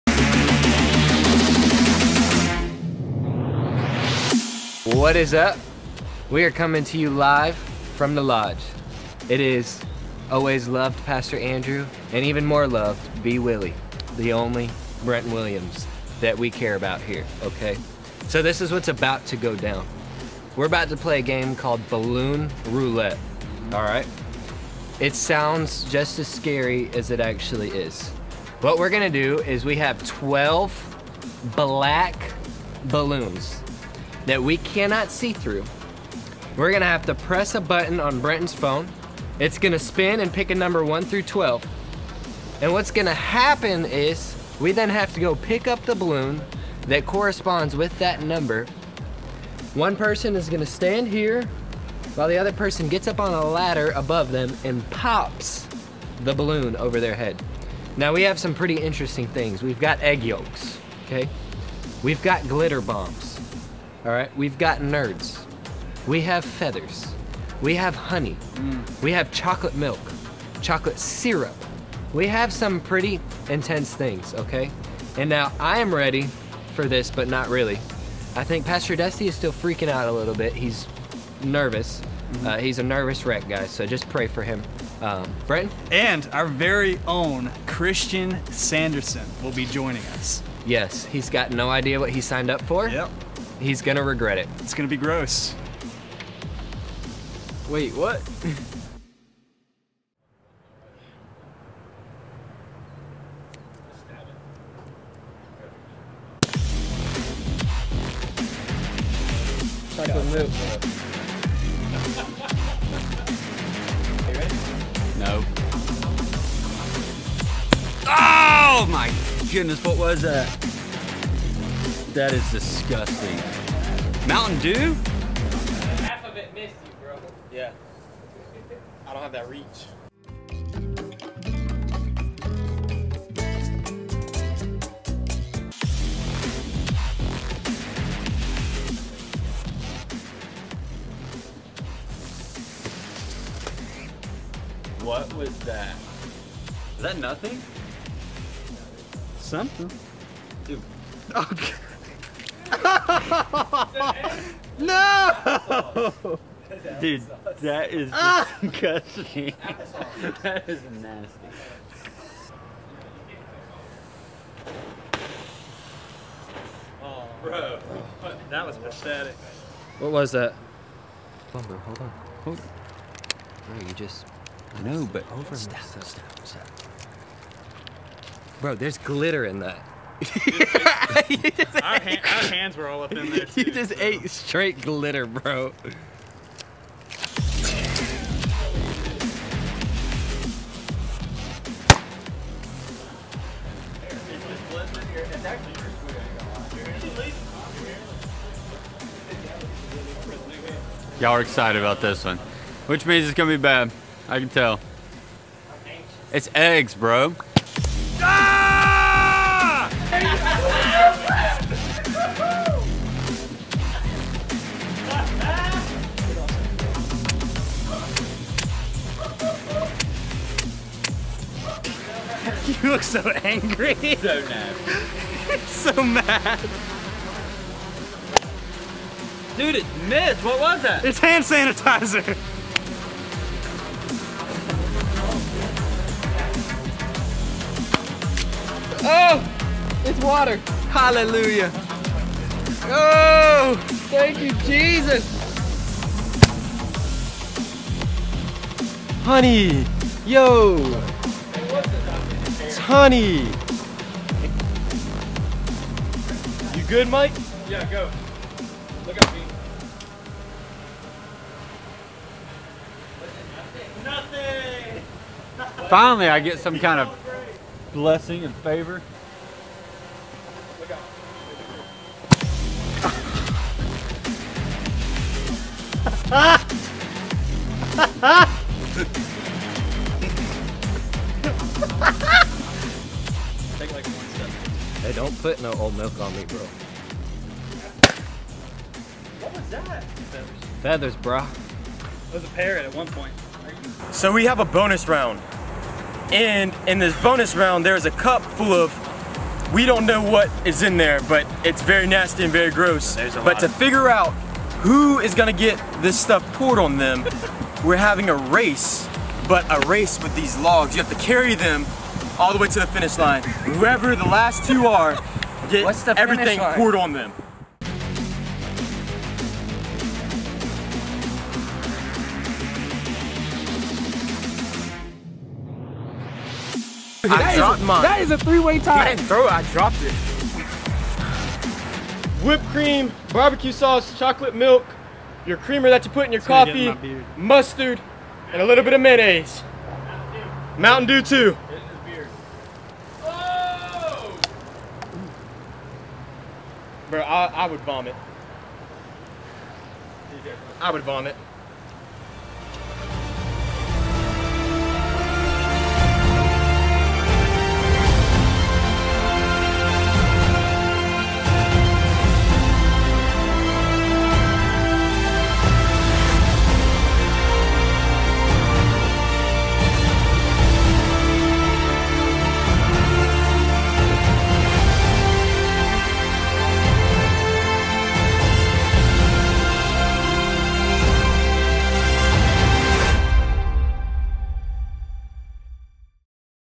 Balloon Roulette 😎🔥
12 Balloons, 12 substances, 4 dudes.